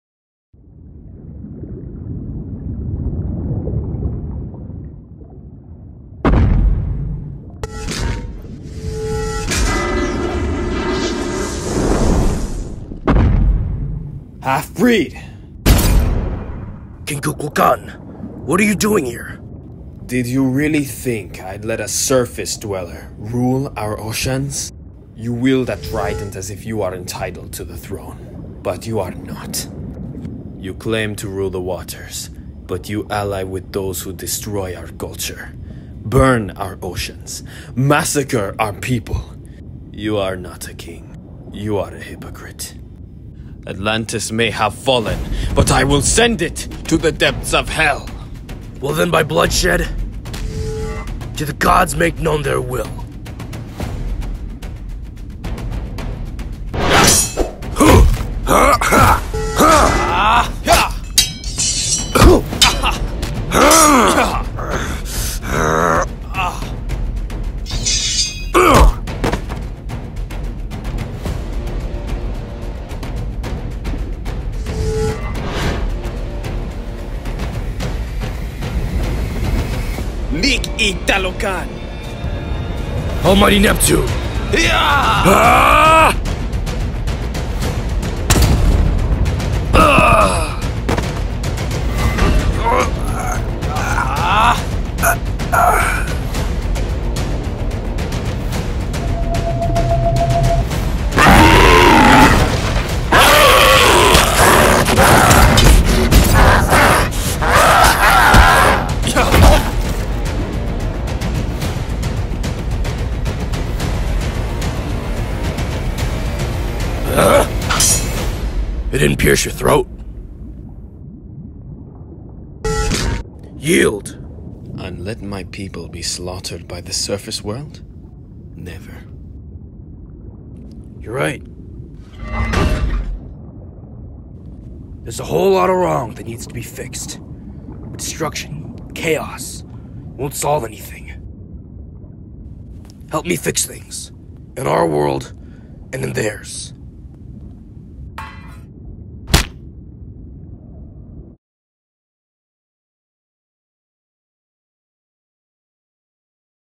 AQUAMAN VS NAMOR: Battle of the Kings (Marvel/DC Stop-Motion Figure Fight)
Arthur Curry, the Aquaman, faces off against Talokan’s very own hero of the deep.. Namor, the Sub-Mariner!

This video took me a while to make, but I really think you’ll enjoy it! Let me know if you guys want more long form content like this.

Frozen Vibranium Productions as Aquaman and Gill-man

​⁠​⁠​⁠​⁠​⁠​⁠​⁠​⁠​⁠@videohousejsq as Namor